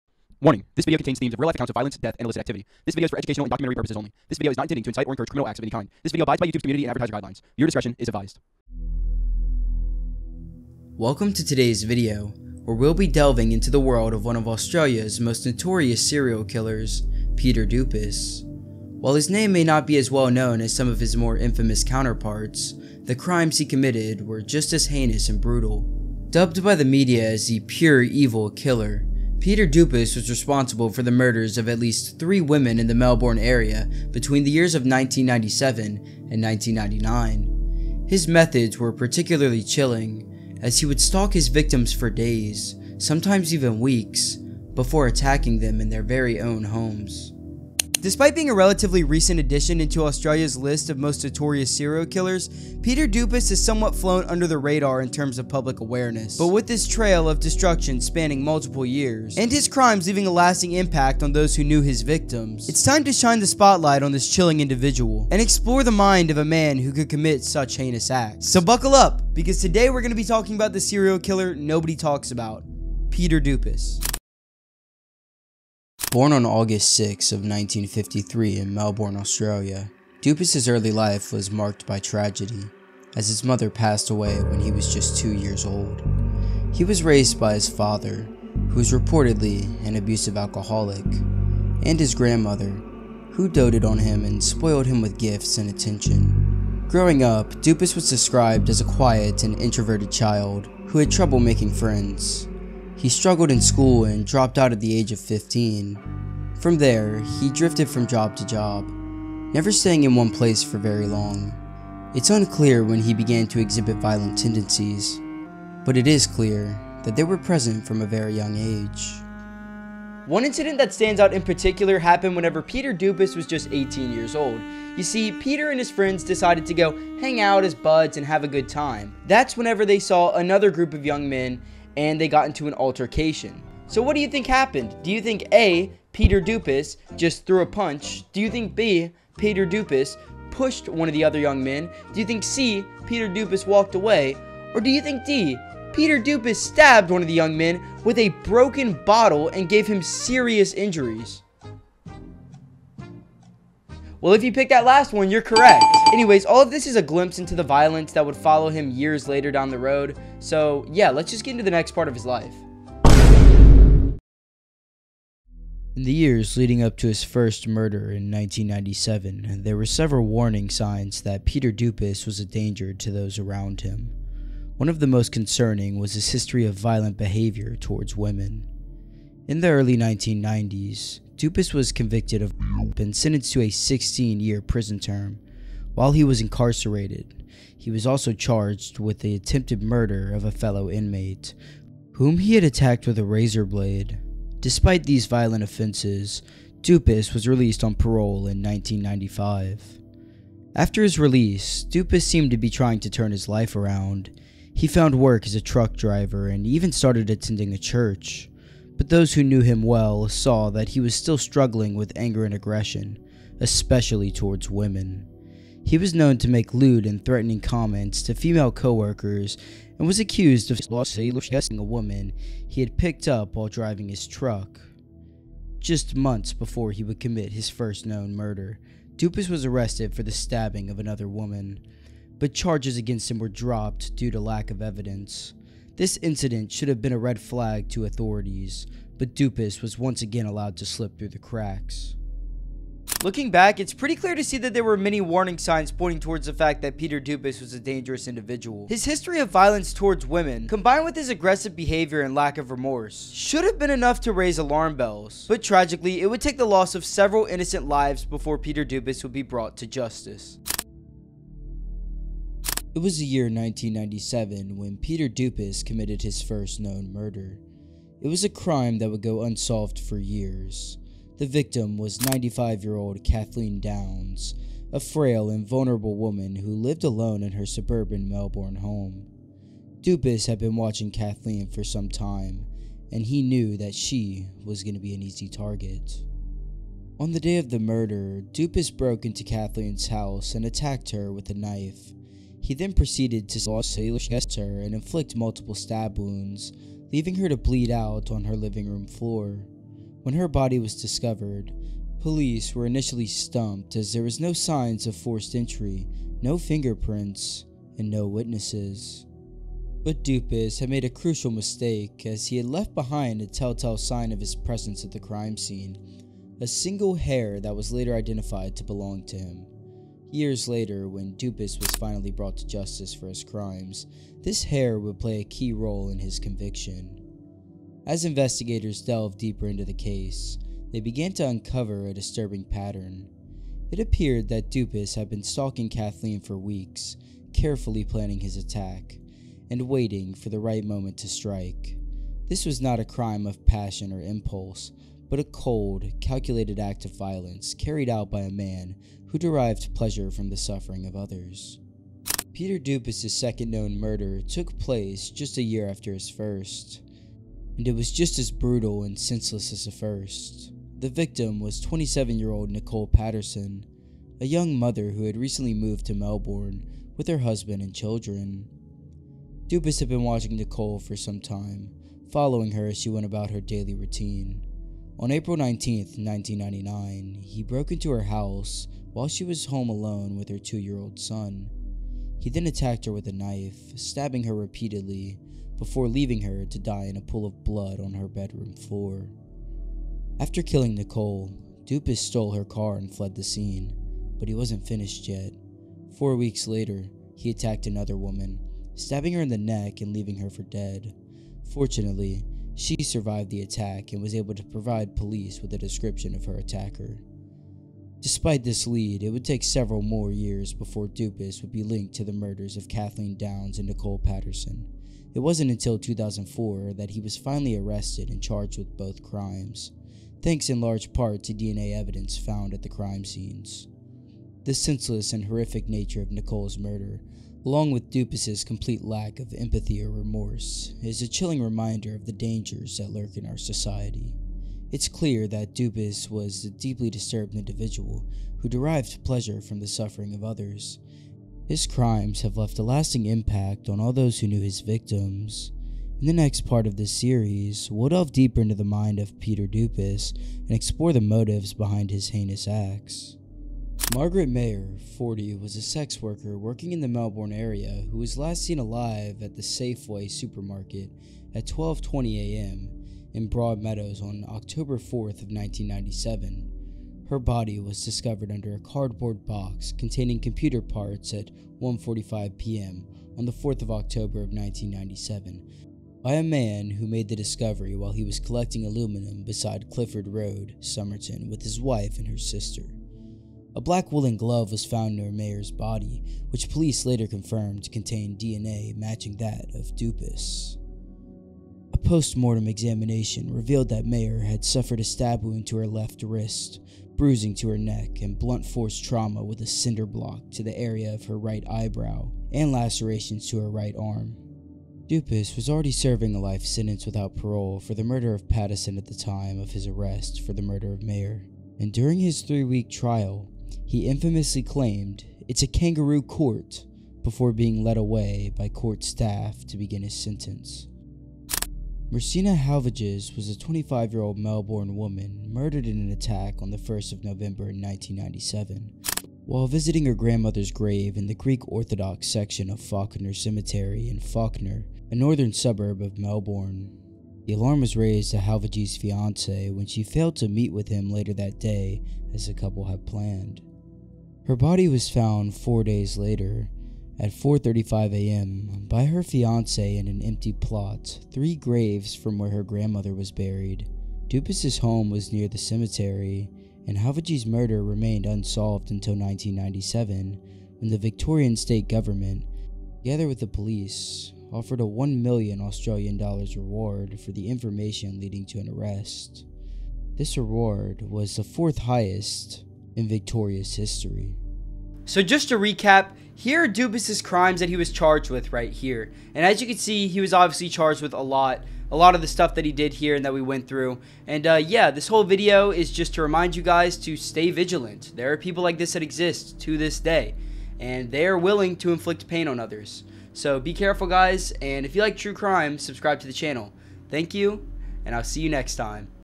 The Serial Killer Nobody Talks About - Peter Dupas
Dupas was an Australian man who was convicted of horrible crimes, and in this video we tell the story of such a twisted individual.